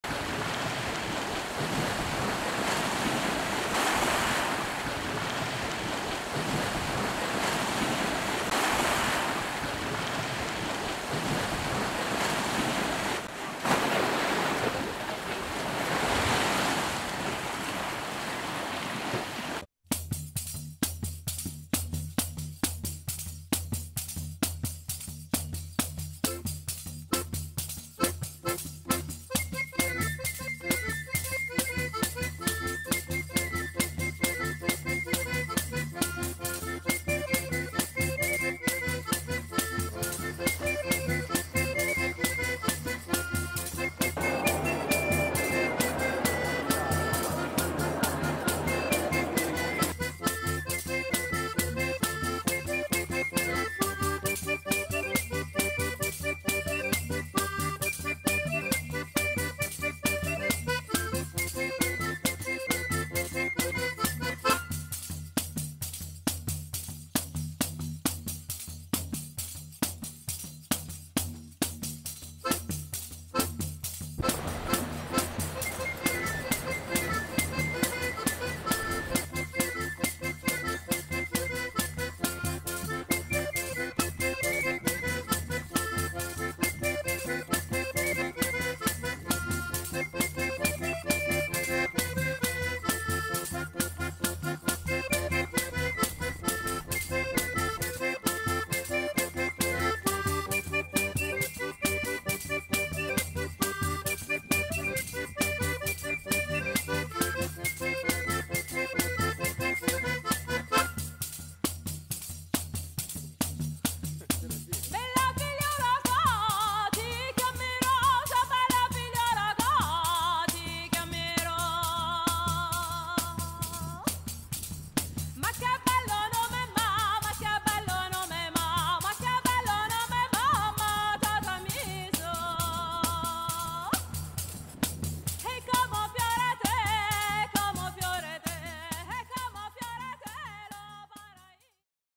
Italy - Magna Graecia (the promised land)
Democracy, Art, Philosophy, Culture, Mathematics, Astronomy, and Medicine sprung to full bloom in this corner of the Mediterranean